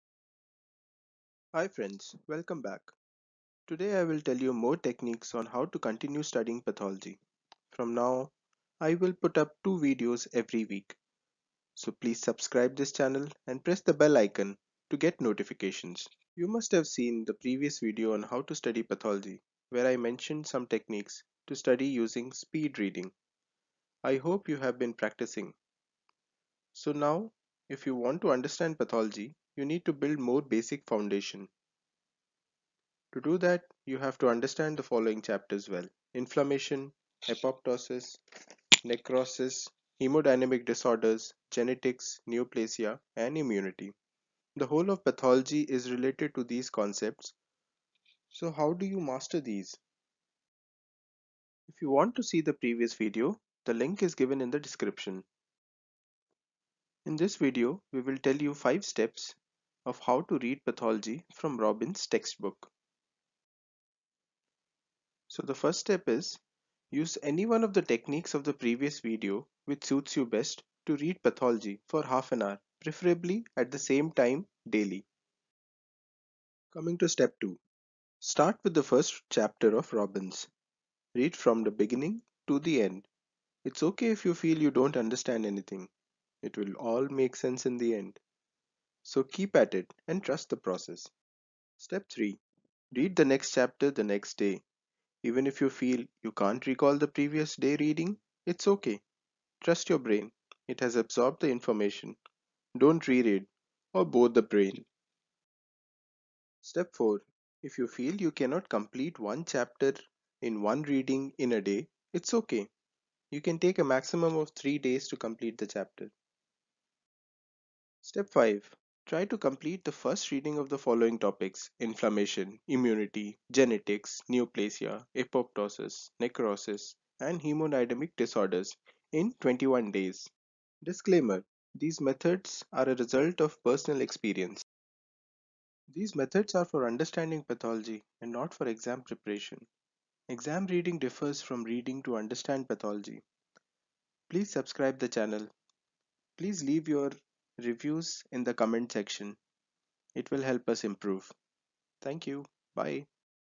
Watch this before opening pathology text book!!
how to study pathology, how to study pathology in mbbs, how to study pathology from robbins, pathologyriddles, This video is a continuation of how to study pathology to understand the basics.
Pathology is considered very difficult by 2nd year students of all the health science branches. So this is an attempt to make it simple. Pathology is the foundation of medicine. So if the basics are not strong, it becomes very difficult to explain medicine.